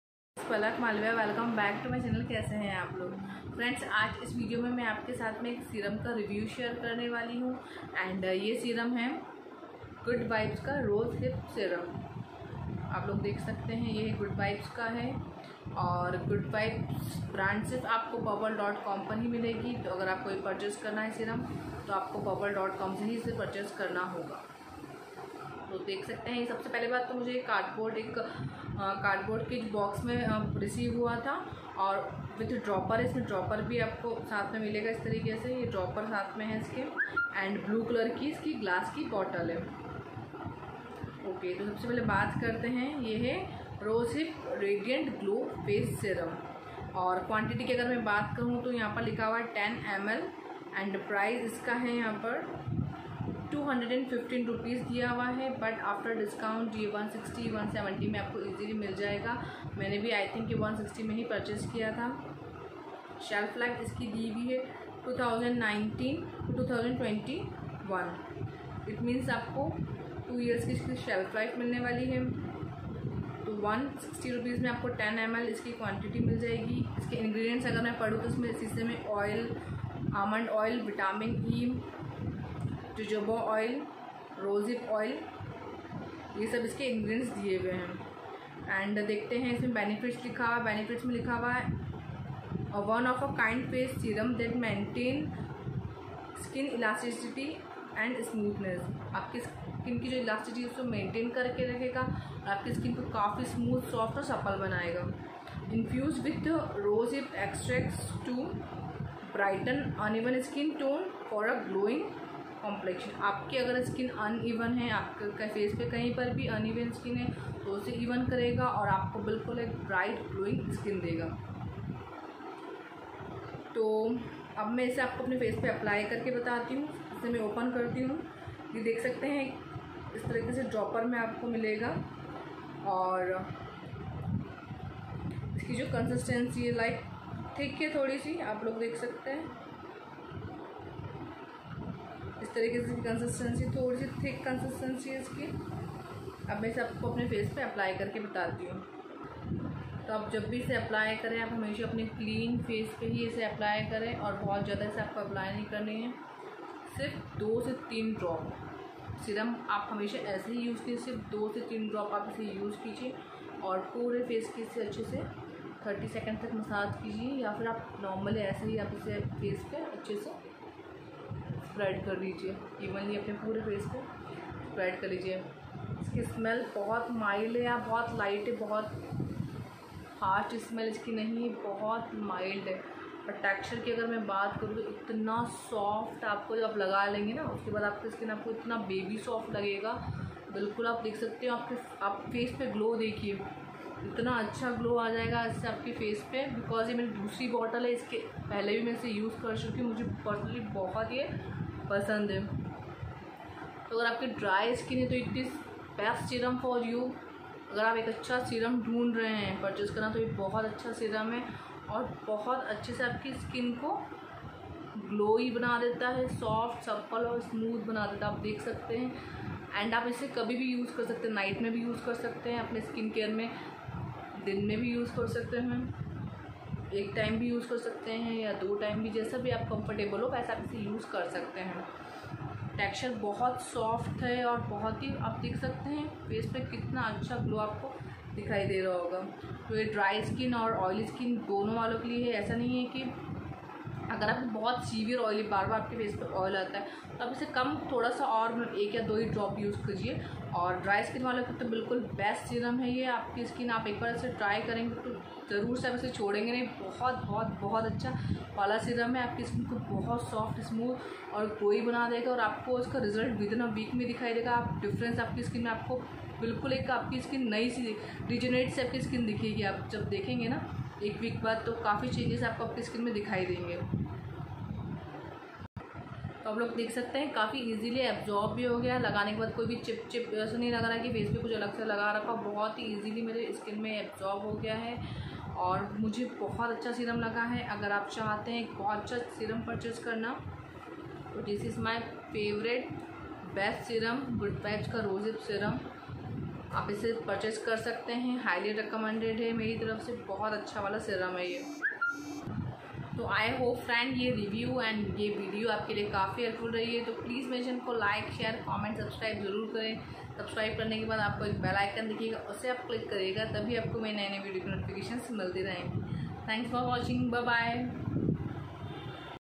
Good Vibes Rosehip Radiant glow face serum ll honest review ll best serum ll Palak Malviya ll
hi friends this is Palak Malviya welcome back to my channel kaise hain aap log friends aaj is video mein main aapke sath mein good Vibes rosehip radiant face glow serum ka review karne wali yah serum best dry skin walon ke liye agar aapki skin Par glow nahin hai dull rahati aapke skin to yah serum bahut achcha aap ke face par glow dega aap ki skin ko soft smooth and supple banaega oily skin Wale bhi ise kam quantity mein use kar sakte hain one of the best serum hai iska purchase link discription box mein. please like share subscribe my channel